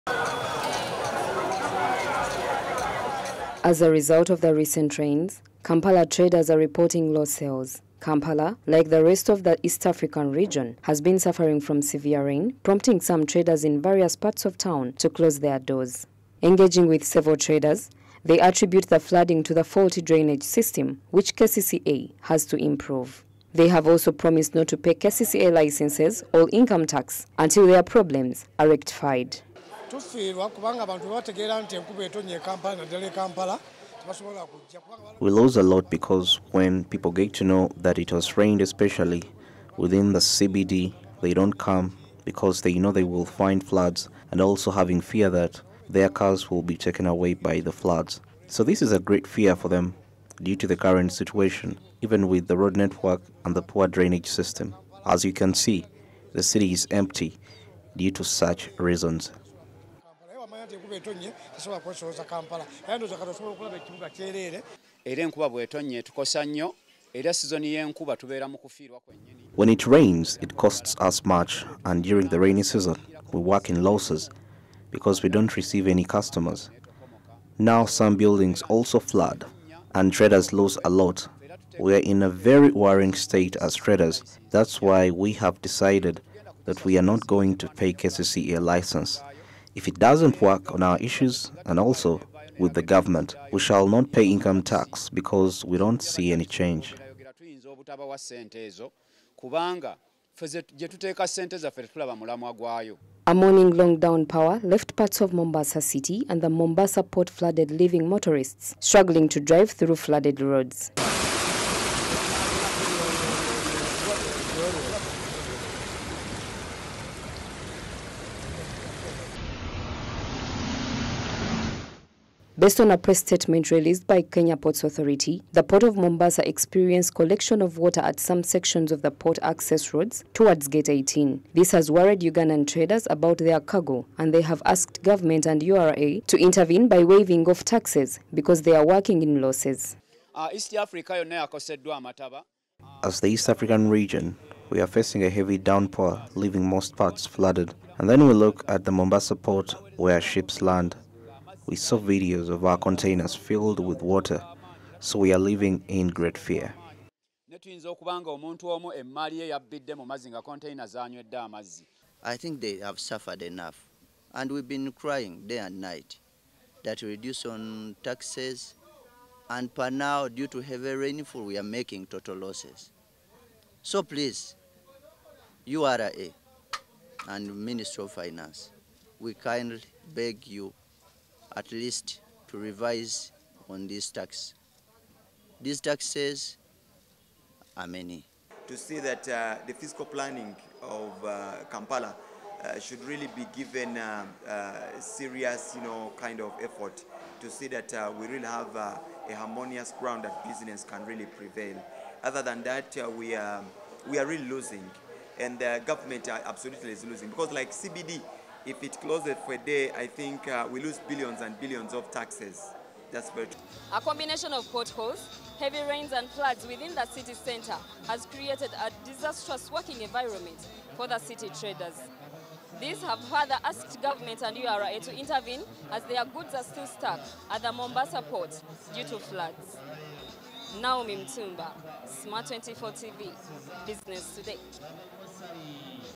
EAC FLASH FLOODS
Potholes, relentless downpour rain, and flooding in the nation’s capital create an
awful working environment for city traders. They acknowledge that the
government has reduced the amount of water that enters arcades, including
Qualicel and Sekaziga, are still negatively affected. The same unrest is present in
Mombasa, which has prompted the businessmen to request tax exemptions from
the government in order to get their goods from Mombasa